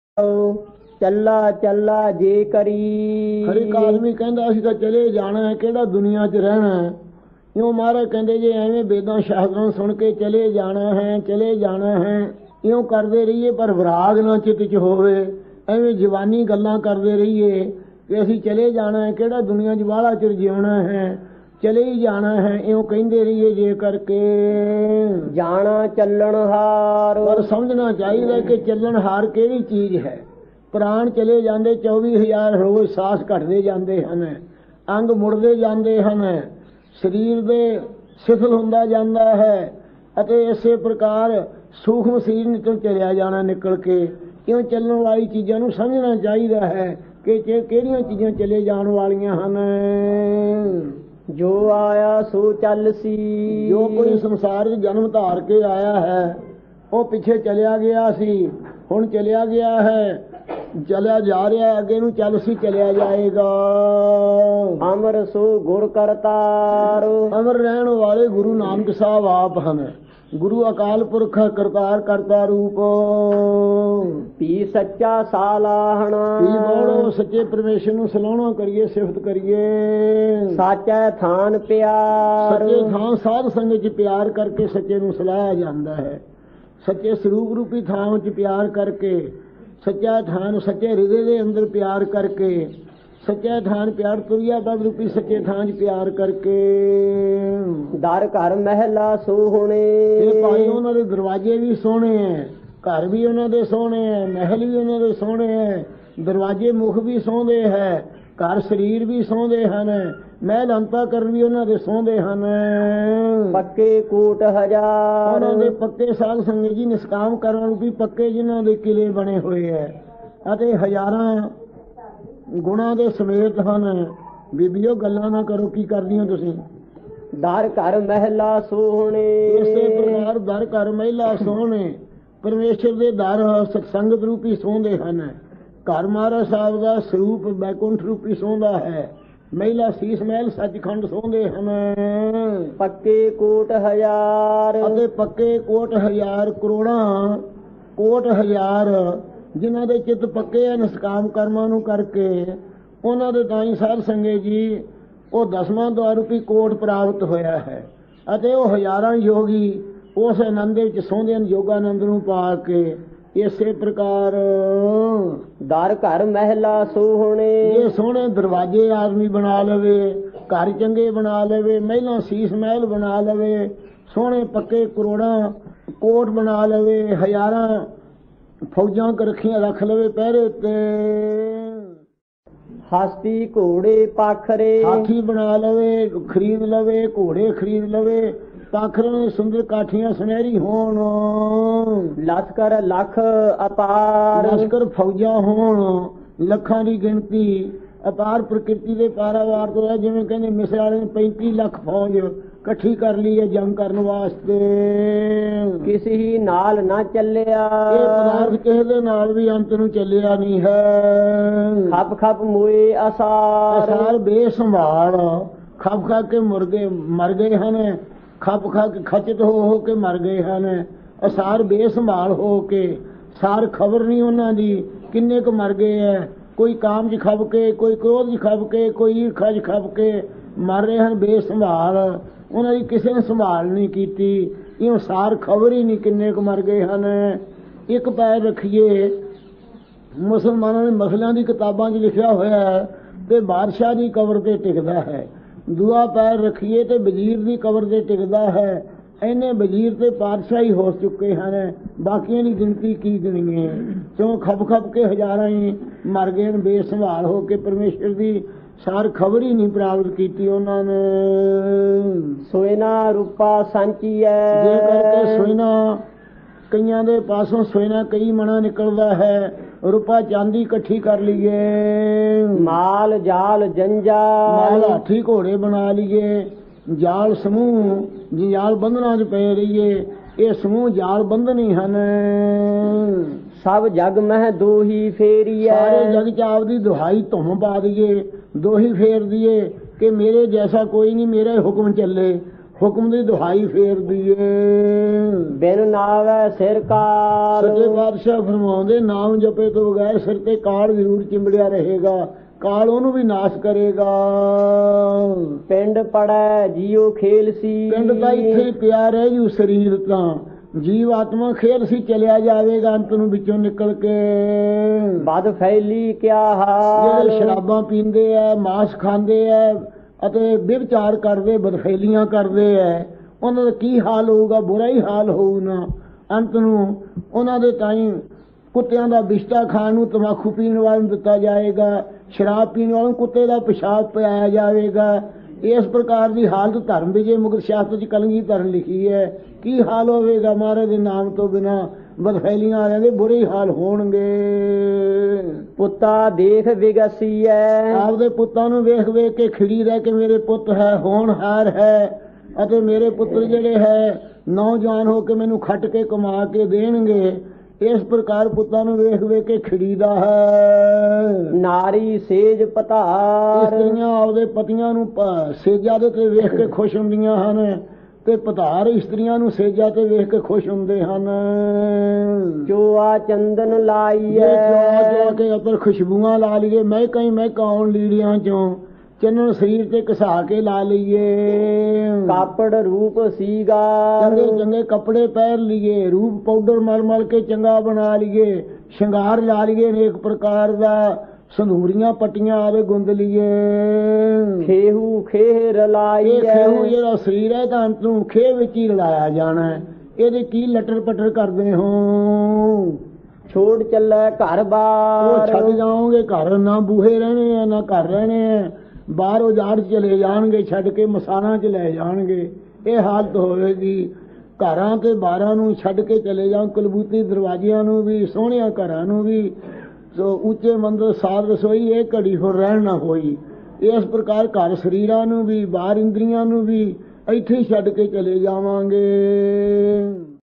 Challa Challa/ I Am Going I Am Going| Sant Gurbachan Singh Ji Khalsa Bhindranwale
Sant Gurbachan Singh Ji Khalsa doing katha from Sri Guru Granth Sahib Ji Maharaj